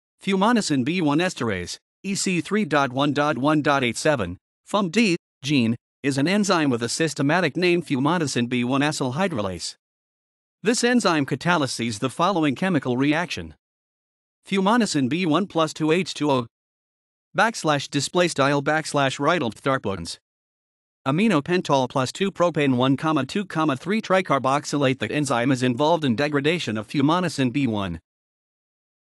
Fumonisin B1 Esterase
Fumonisin B1 esterase (EC 3.1.1.87, fumD (gene)) is an enzyme with a systematic name fumonisin B1 acylhydrolase. This enzyme catalyses the following chemical reaction

fumonisin B1 + 2 H2O

    {\displaystyle \rightleftharpoons }
   aminopentol + 2 propane-1,2,3-tricarboxylateThe enzyme is involved in degradation of fumonisin B1.